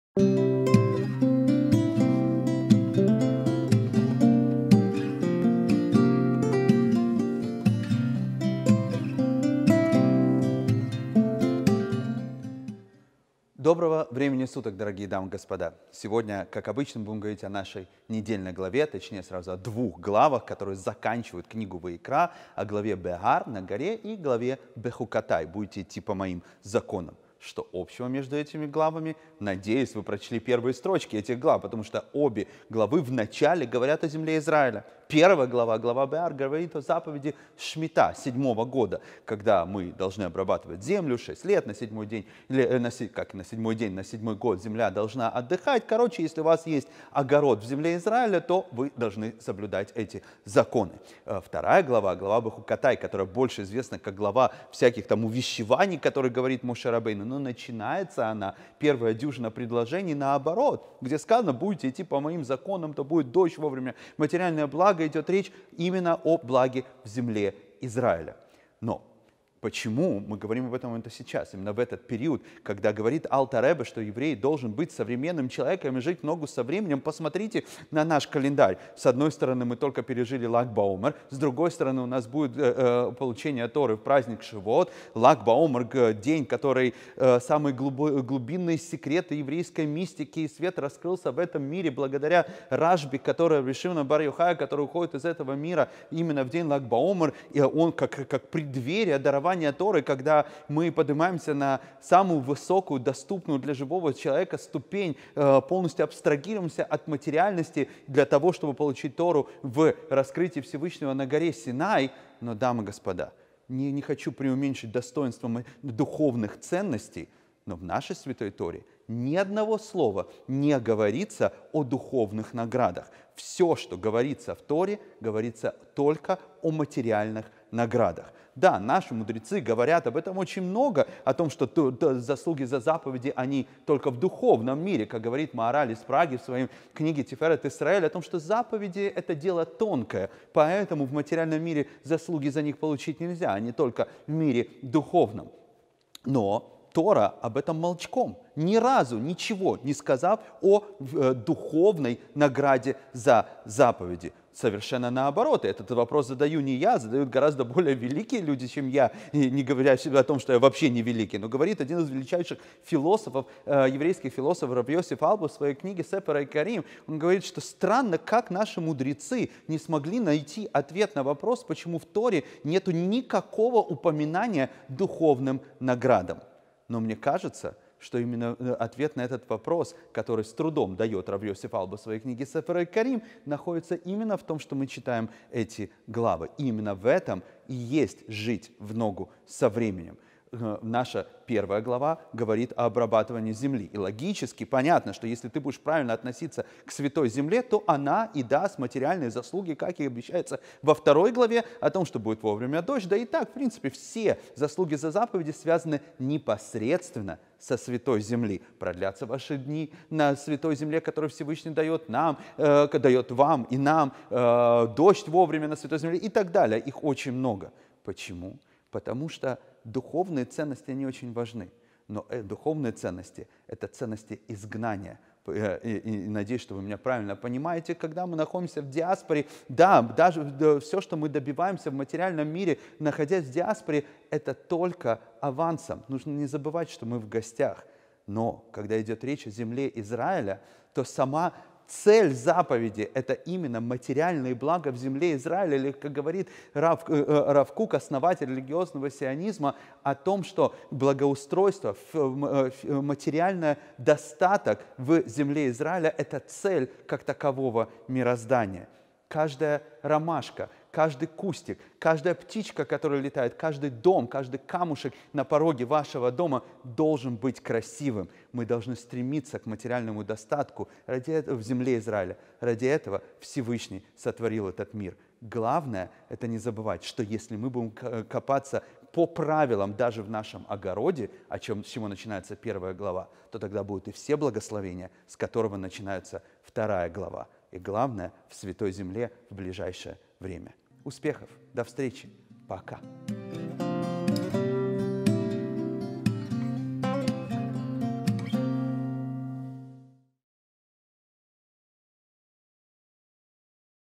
🌱Огородники. Беар - Бехукотай 5781 | Недельная глава с равом Альтшулем
Доброго времени суток, дорогие дамы и господа!

Сегодня, как обычно, мы будем говорить о нашей недельной главе - вернее, сразу о двух главах, которые заканчивают книгу Ваикра - о главе Беар ("На горе") и главе Бехукотай ("Будете идти по Моим законам").

Что общего между этими главами? Надеюсь, вы прочли первые строчки этих глав, потому что обе главы в начале говорят о земле Израиля...